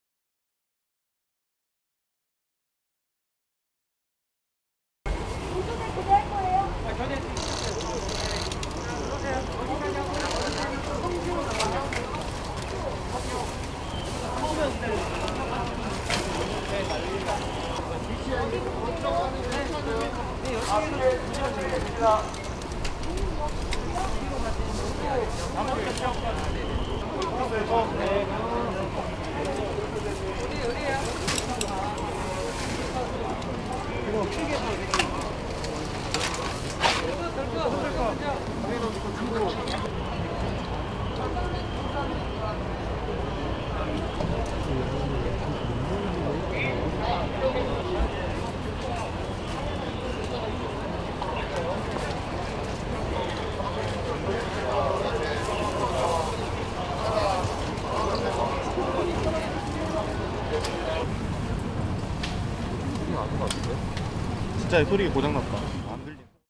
Seoul stampede: Emergency workers move bodies from site of deadly crush
Emergency workers in South Korea's capital carry bodies away from the scene of a stampede that has so far claimed at least 151 lives.
The victims of the stampede are mostly teenagers and adults in their 20s, according to officials.
More than 100 were injured during the incident after being crushed by a large crowd pushing forward on a narrow street during Halloween festivities in the Itaewon area of Seoul.